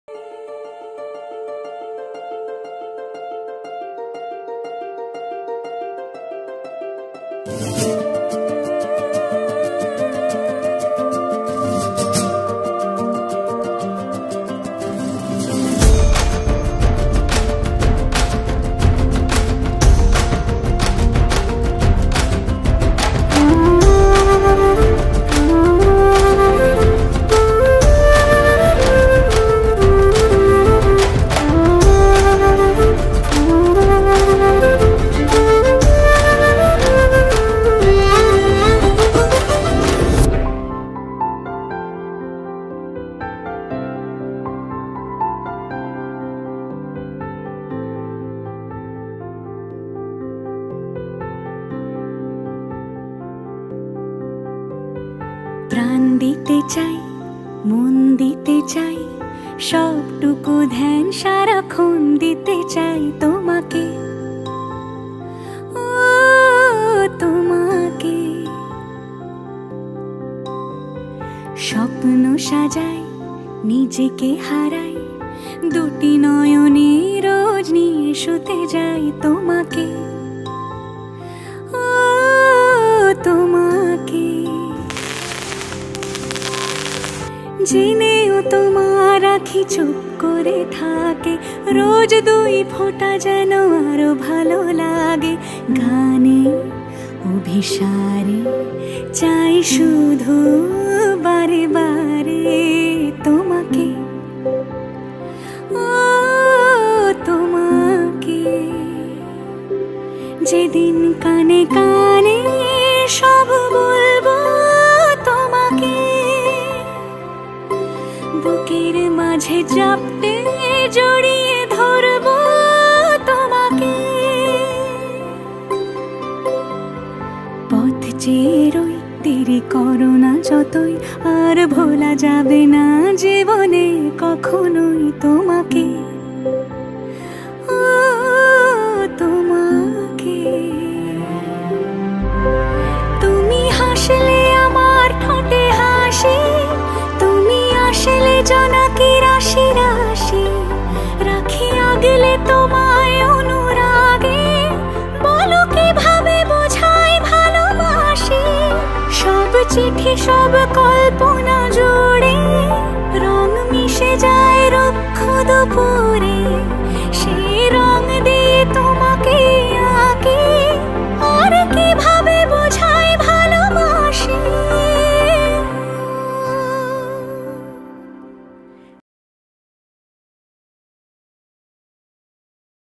Best Bengali wedding film teaser of Arindam weds Piyali in Garhbeta
Most natural couple I  have ever seen.